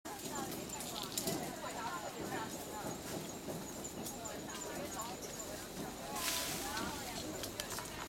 三歲雌猴抱出生嬰猴
此嬰猴是其妹妹